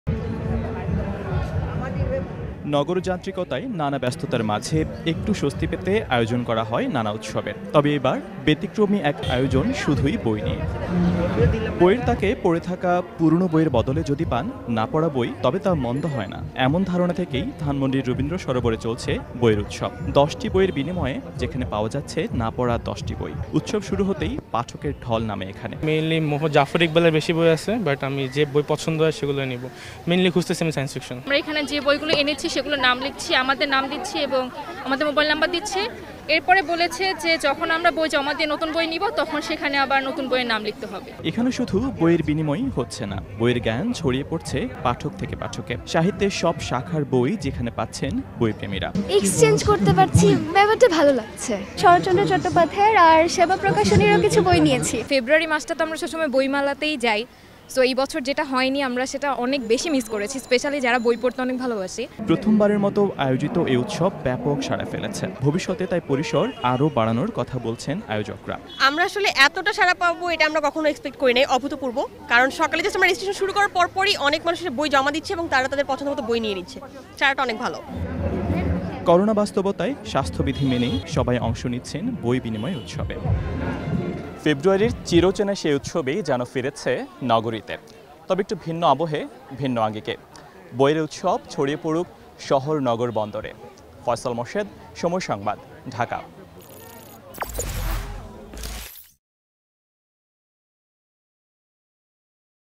বই পাল্টাপাল্টি নিয়ে ব্যতিক্রমী বই উৎসব! | Book Festival | Somoy TV
ভাষার মাস ফেব্রুয়ারি মানেই বইয়ের উৎসব। যদিও, বাংলা একাডেমি বা সোহরাওয়ার্দীতে নয়, ধানমণ্ডির রবীন্দ্র সরোবরে হয়ে গেলো ব্যতিক্রমী বই উৎসব। যেখানে দশটি পুরানো বইয়ের বদলে মিলেছে নতুন না পড়া বই। সকাল দশটায় শুরু হয়ে উৎসব চলে বিকেল ৫টা পর্যন্ত।